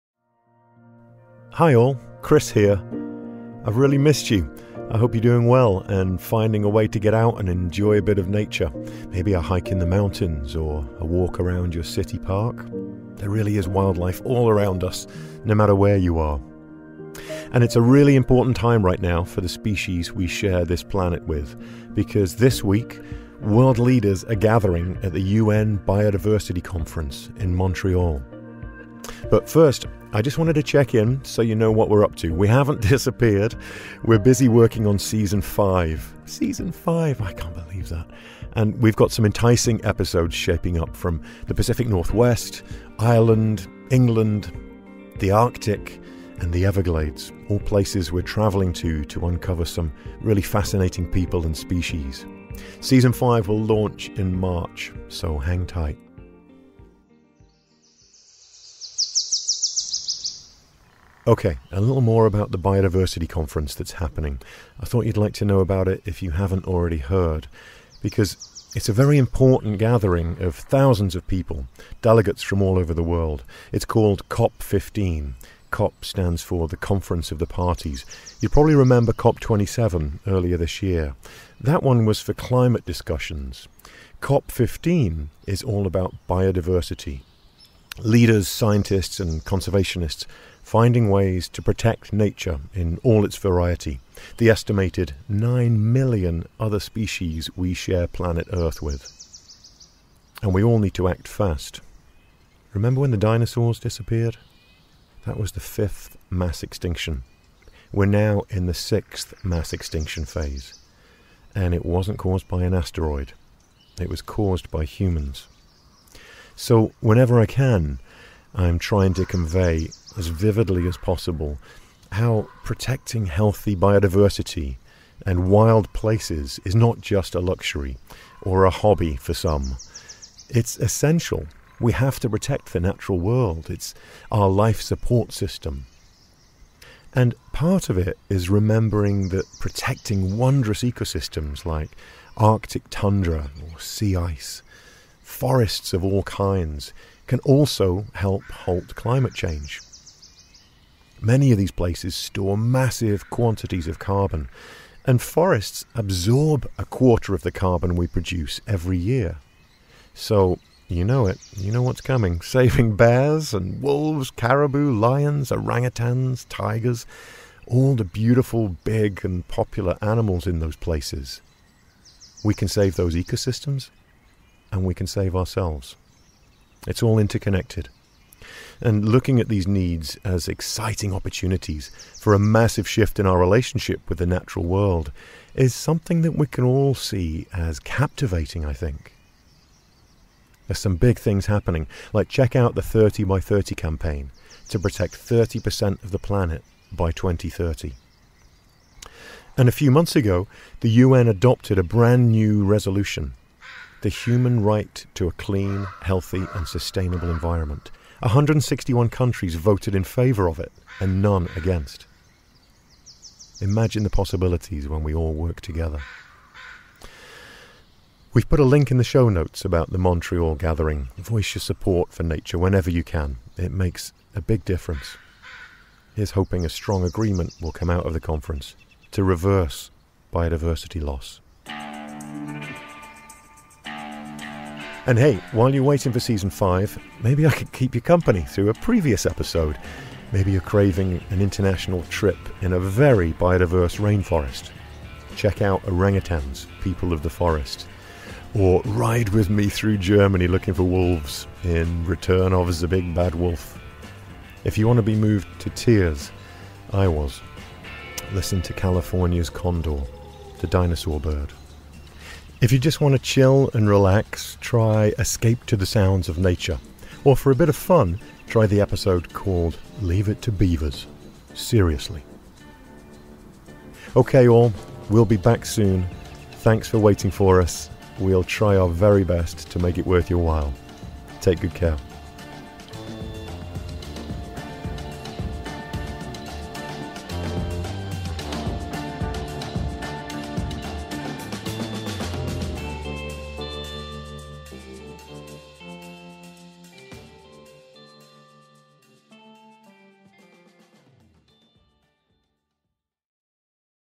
A short check-in from Chris
Hi all - Chris here - I’ve missed you! I hope you’re doing well and finding a way to get out and enjoy a bit of nature….maybe a hike in the mountains, or a walk around your city park? There really is wildlife all around us, no matter where you are. We're busy working on Season 5 (! wow can’t believe that!). and we’ve got some enticing episodes shaping up from the PNW, Ireland, England, the Arctic, and the Everglades! All places we're traveling to to uncover some really fascinating people and species. Season 5 will launch in March, so hang tight.

And it’s a really important time right now for the species we share this planet with. Because this week, world leaders are gathering at the UN Biodiversity Conference in Montreal. You can learn more about the conference by following this . Also, a great video link just out from the Guardian that is about everything I talk about and COP!

Nature sounds in this episode provided by Gordon Hempton, the Sound Tracker